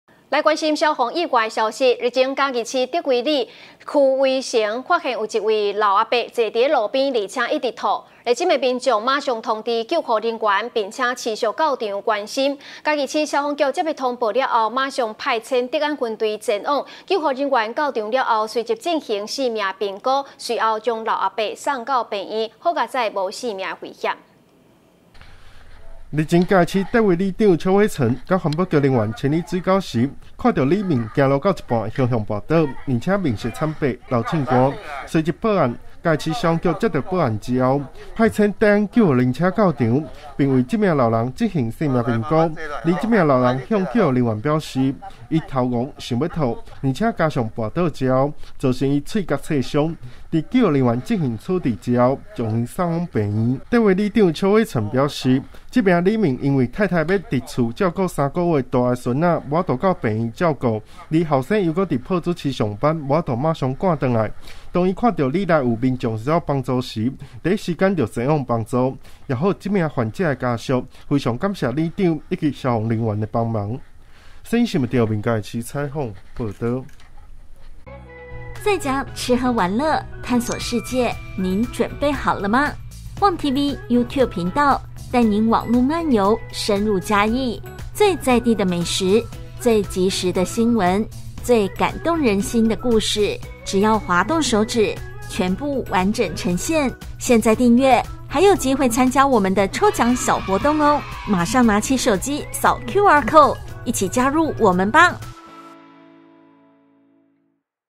世新新聞 老翁突倒路邊 店家民眾救援CALL救護車送醫
111年5月20日 (五) 世新新聞

老翁突倒路邊 店家民眾救援CALL救護車送醫

日前嘉義市竹圍里邱威誠發現一名老翁坐在路邊而且不斷嘔吐，熱心民眾馬上通知救護人員，並持續在場關心，嘉義市消防局接獲通報後，馬上派遣德安分隊前往，救護人員到場後，這名老翁臉色慘白，而且冒冷汗，救護人員隨即進行生命評估，隨後將老翁送往醫院，所幸到院後無生命危險。